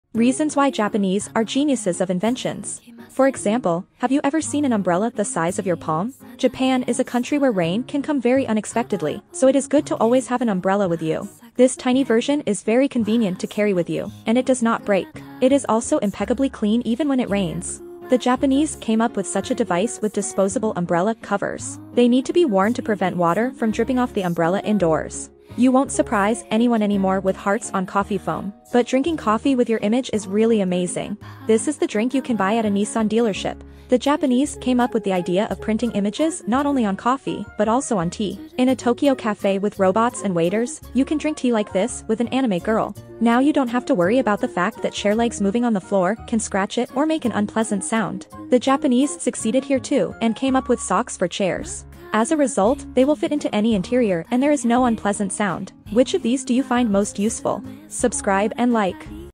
Reasons why the Japanese are geniuses of inventions 😱japan
Reasons why the Japanese can be called geniuses. Amazing Japanese Inventions